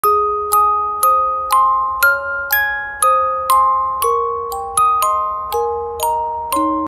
Aww Adorable Red Panda
Cute Little Guy He Loves to Sleep during the day and be lazy comes out mostly at night time 🦊🦊🦊